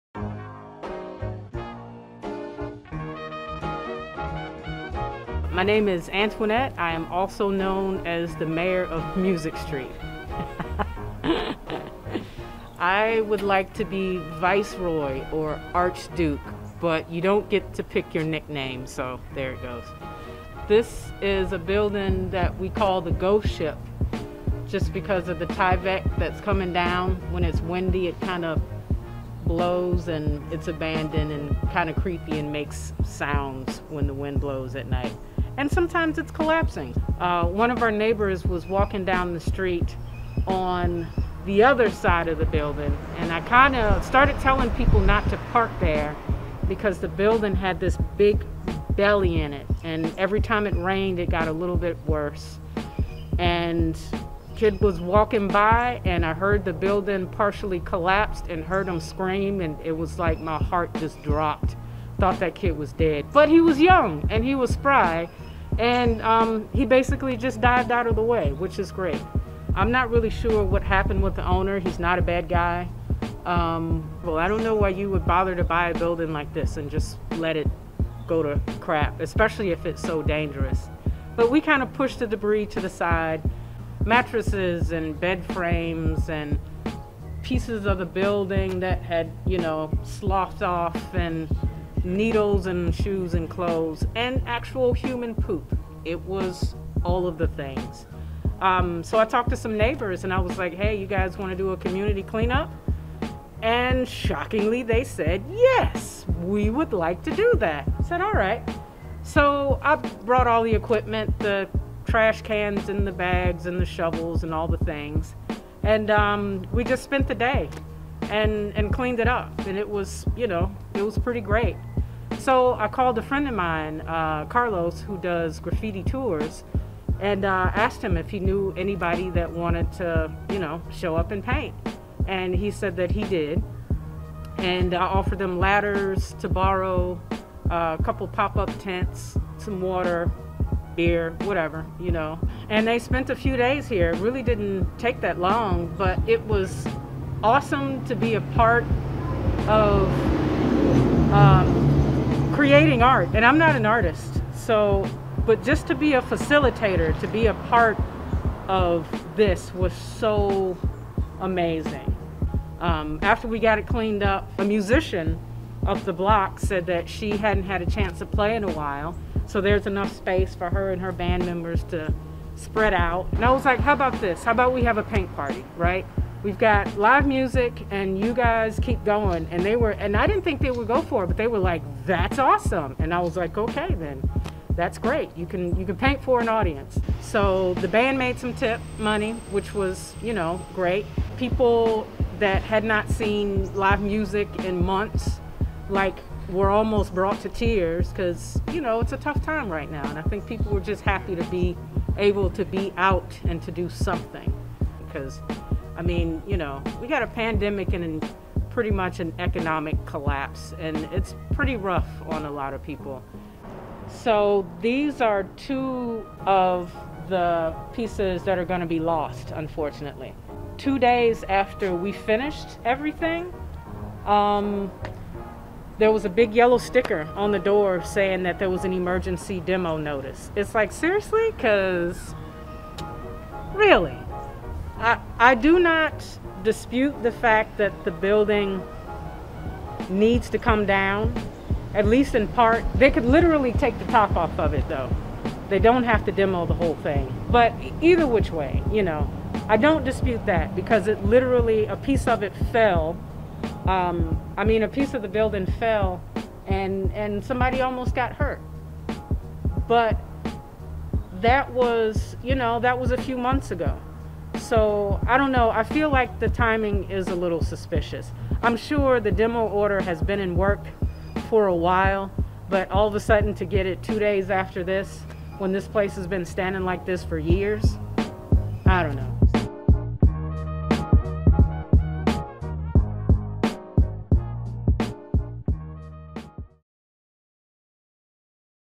New Orleans Murals - Music Street "Ghost Ship"
"Recently, several artists started creating murals on a blighted building in the 9th Ward neighborhood, dubbed 'The Ghost Ship'. It’s been dangerously crumbling & a few beams almost took a guy out while he was walking by. For years, the Music St neighbors have been trying to get the city & absentee owner to do something about the building.⁣⁣
Finally, one of the neighbors (who is lovingly dubbed Mayor of Music St) began recruiting artists to paint murals on the building. It’s transformed from an eyesore to an art installation.⁣
The day after the 'Mardi Gras Indian Queen' mural (@ 2:30) was completed, there was an Emergency Demolition notice hung on the building by the city.
Check out the beautiful Ghost Ship at Music & Urquhart before it sails away."

ARTIST INFO Etc.: *(@=Instagram)
⁣-(2:30)  @jnayart and @lillianaguinaga created the mural inspired by Mardi Gras Indian Queens and native Louisiana flora.⁣(2:30)

- corner of building with art by @flo_resolution @sweetolivesilver @jdamenace @touchdown.tdoe
Other art by @mrballoonhandsfu @odessablackmore @sydneycalderonart @sanguineskills @sosh.0 @pentagram_popsicles @Crudethings

Big thanks to the wonderful Mayor on Music for organizing this project @heartpineandsoul .⁣
 @dougmaccash is doing continuous coverage on the project. Read more about it and view more pics on his page and on @nolanews.⁣

Video by Keith Wilkes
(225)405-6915